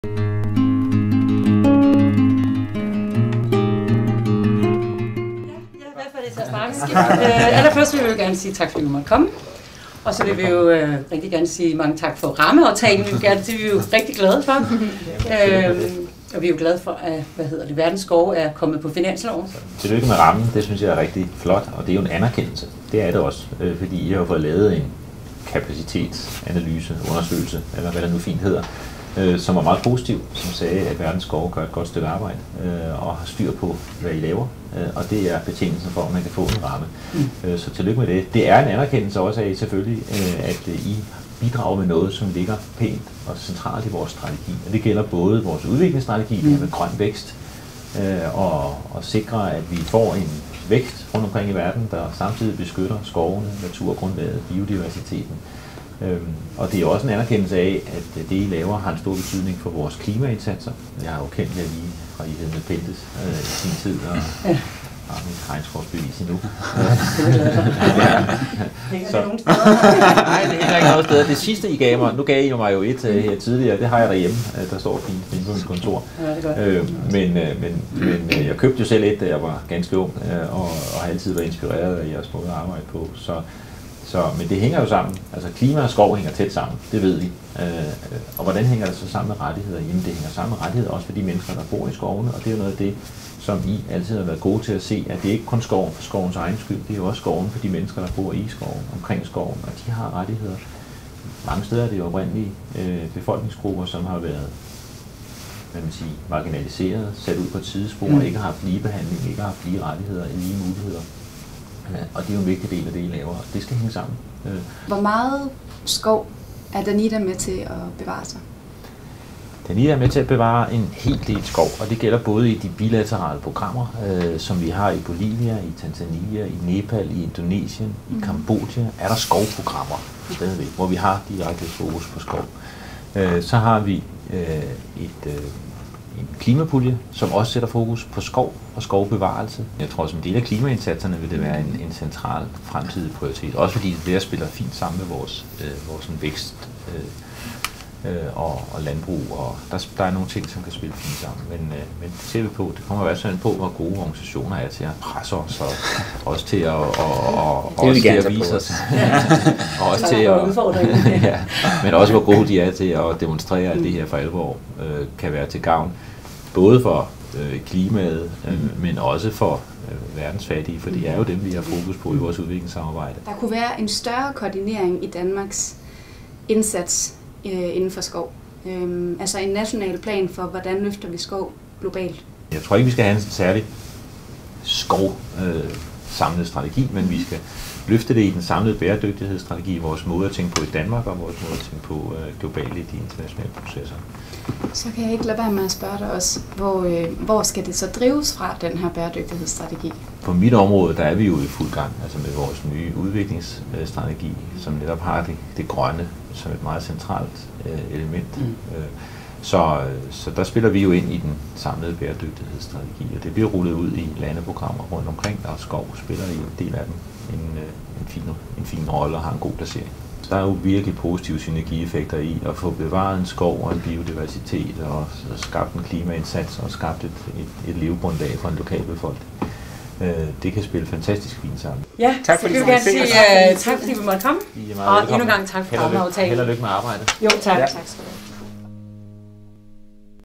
Verdens Skove møder udviklingsminister Christian Friis Bach
Verdens Skove er blevet en rammeorganisation. I den anledning besøgte vi fredag den 12. april udviklingsminister Christian Friis Bach. Her fik vi en snak om aftalen, grøn vækst og bæredygtighedsstrategi.